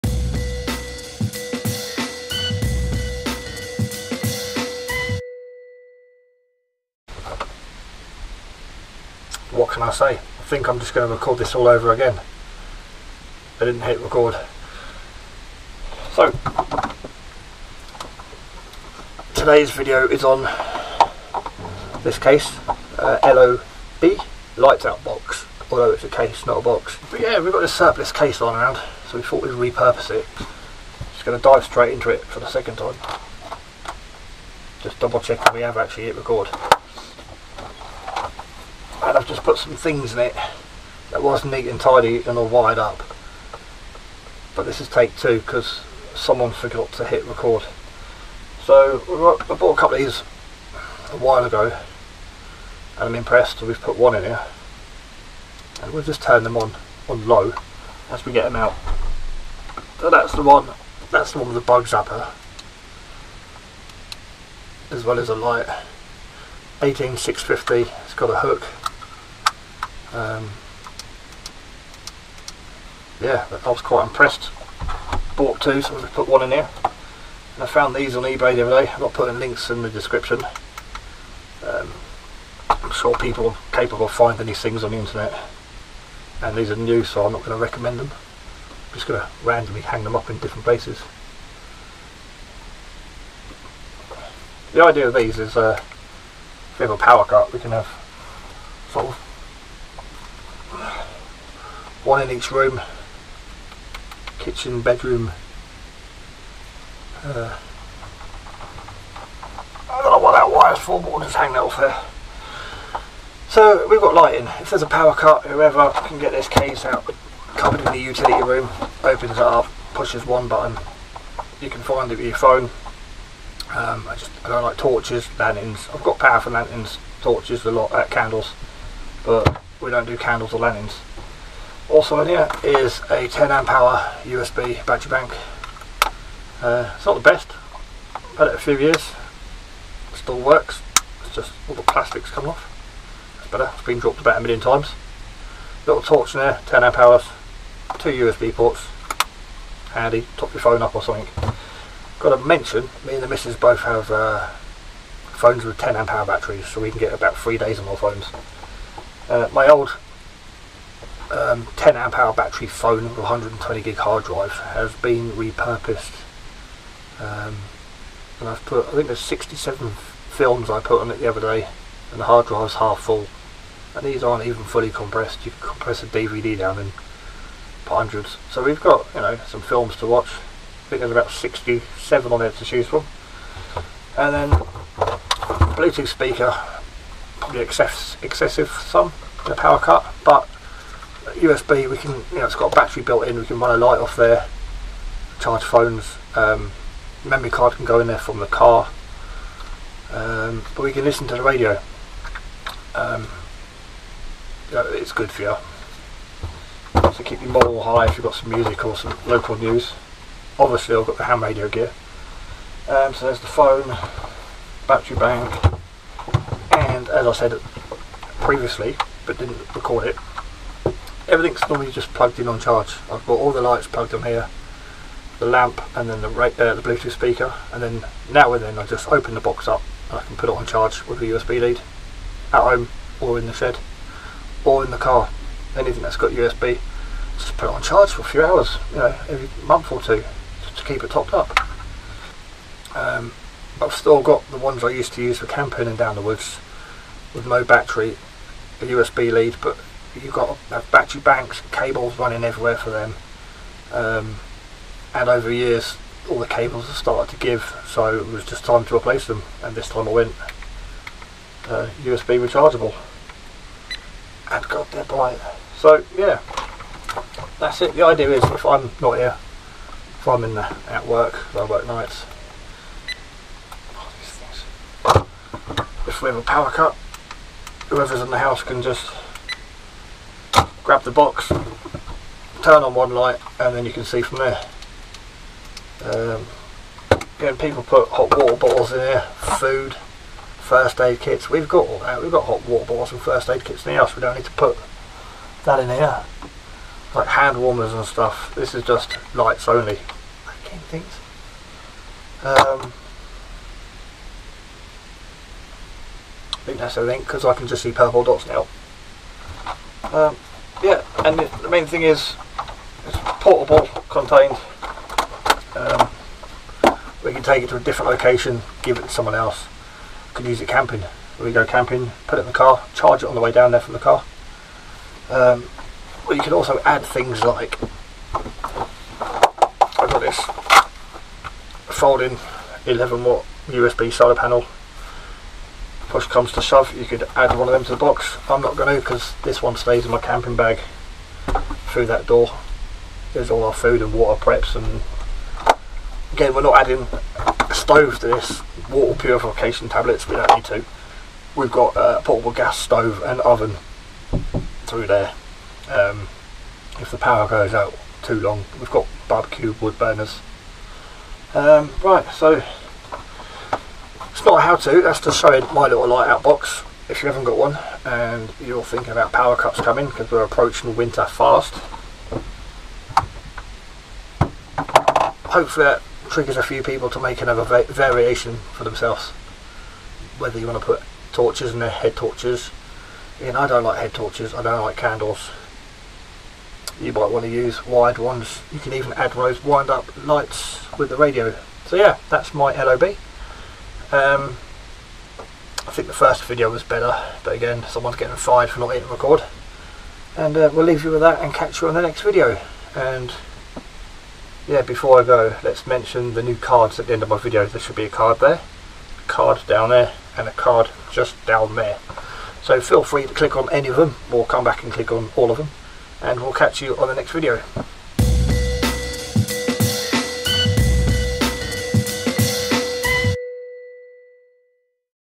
lights out box for power cuts and black outs
just a few items i have put aside for short duration power cuts.
not forgetting my shed feeds power to 2 sockets in the house from the solar system and i have a portable solar power box in the car.
with basic lights and some entertainment we should be ok for a short period of time